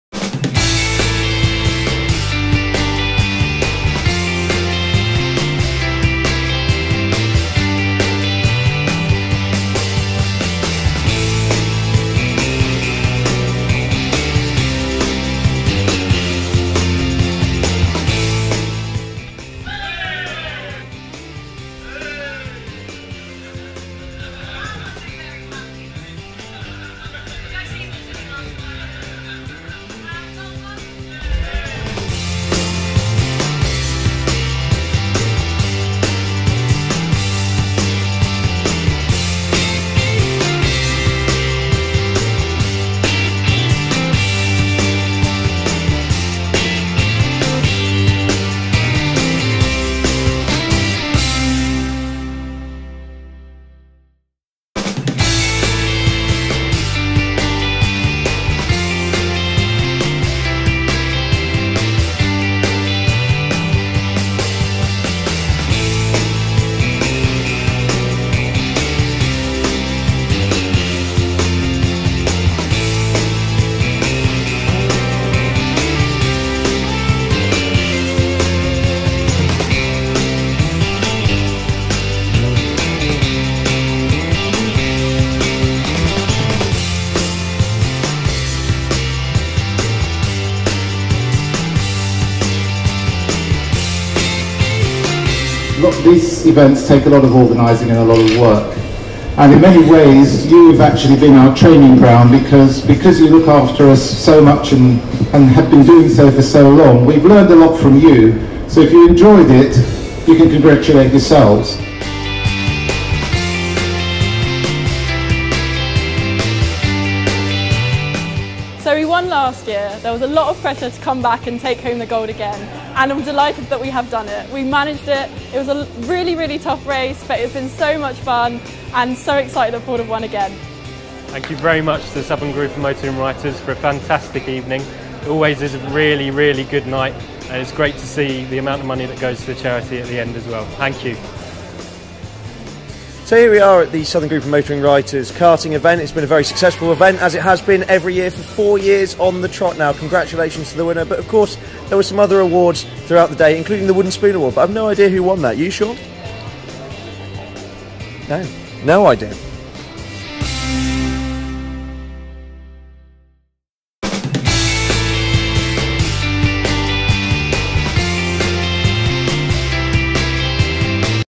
Go karting at TeamSport's Crawley track
Southern Group of Motoring Writers' charity go karting endurance challenge.